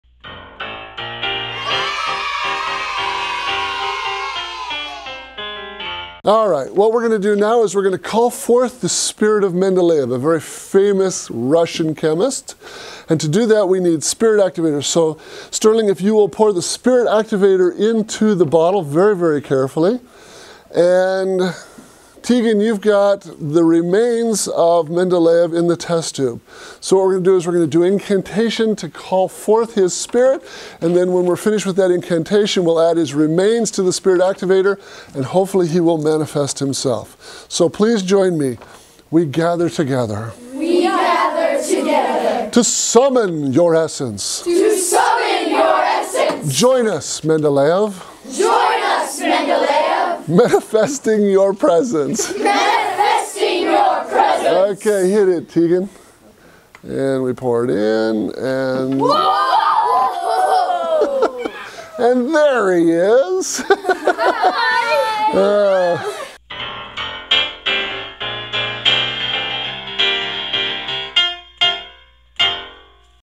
Loose in the Lab with Genie in a Bottle
A cloud of hot steam rises from a 2 liter pop as we call forth the spirit of Mendeleev using his "cremated remains "(manganese dioxide) which are are dumped into the "spirit activator" (concentrated hydrogen peroxide). Water, oxygen, and a substantial amount of heat are released almost instantly and create the illusion that Mendeleev is rising from the bottle to join  your chemistry class.  Very entertaining, easy to do, excellent introduction to colloids, exothermic reactions or catalysts.